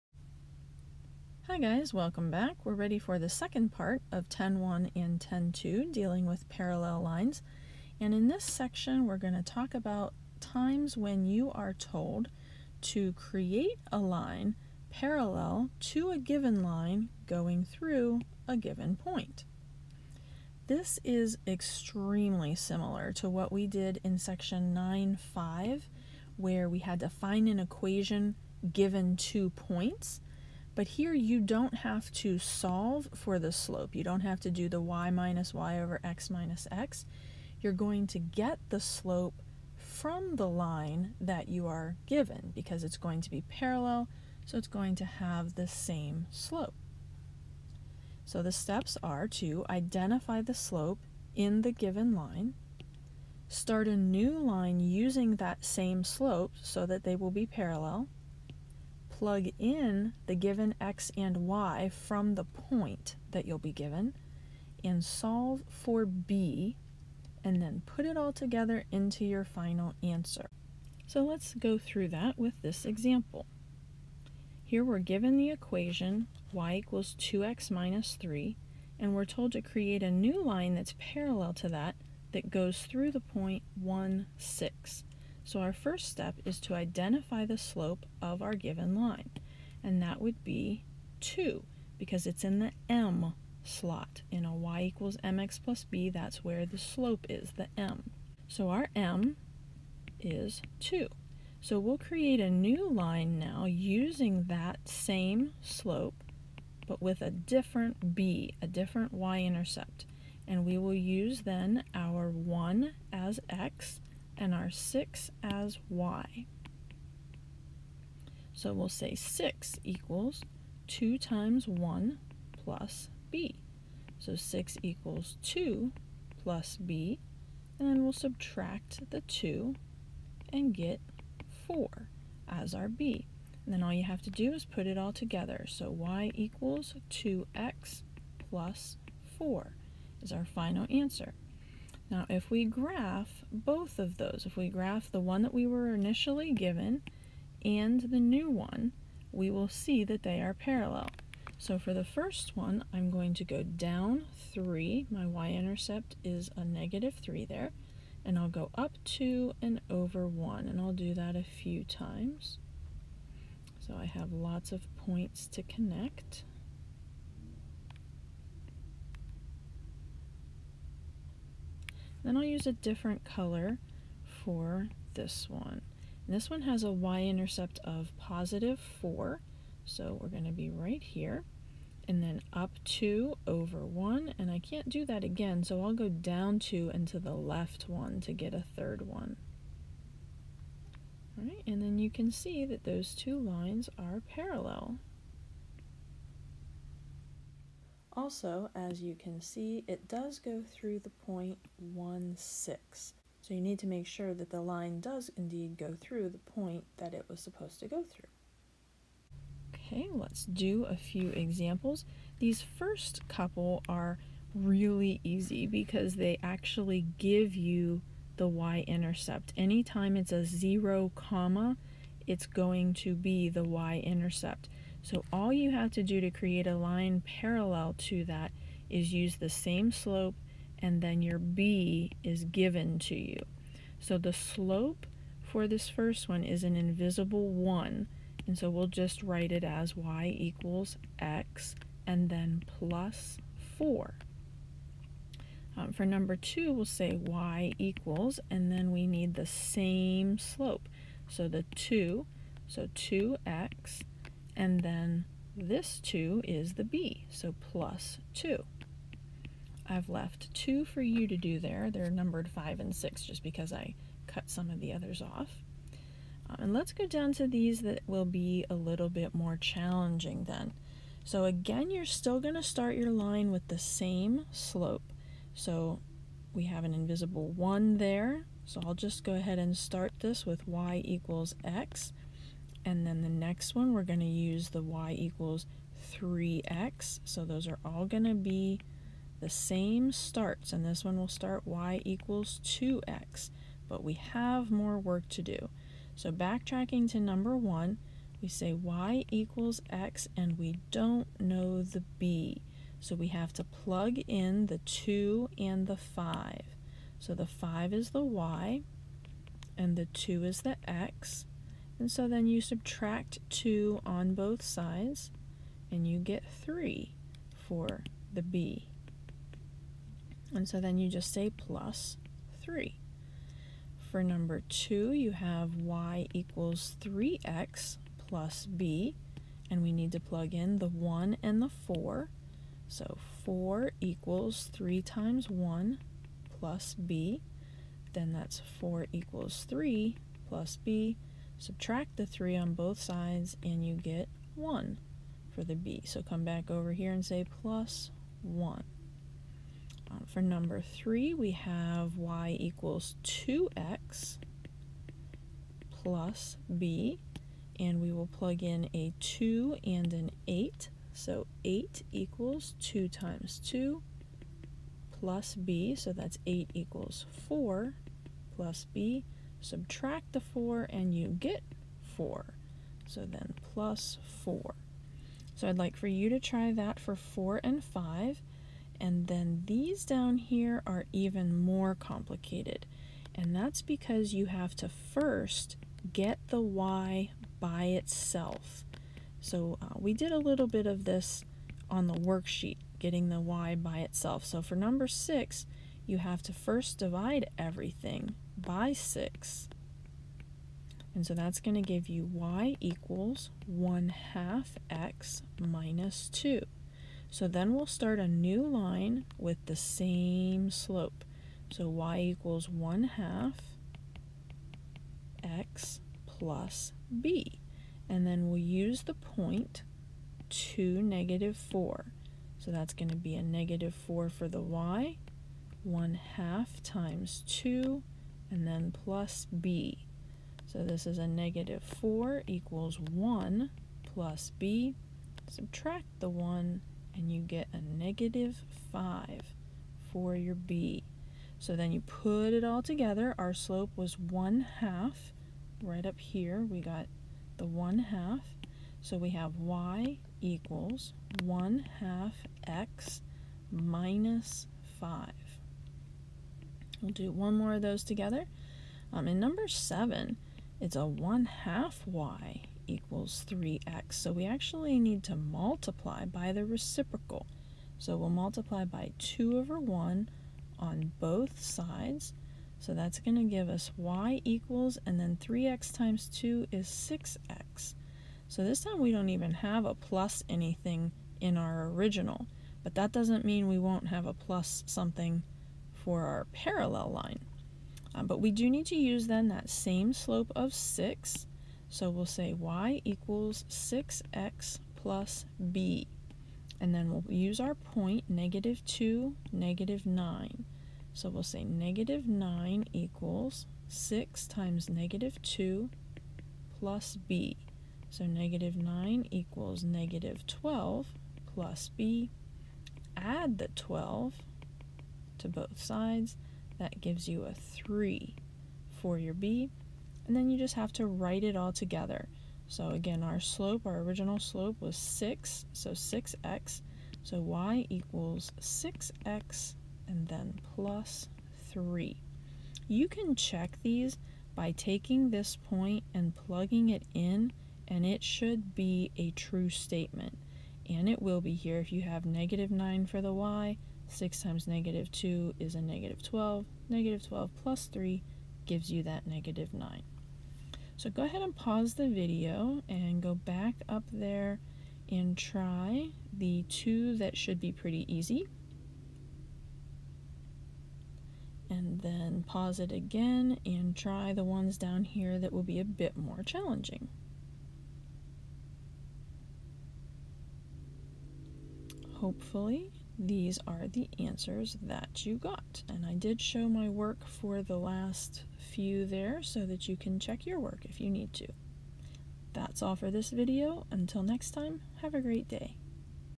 10.1/10.2 Parallel Lines Part 2
In this video we discuss how to create a line parallel to a given line.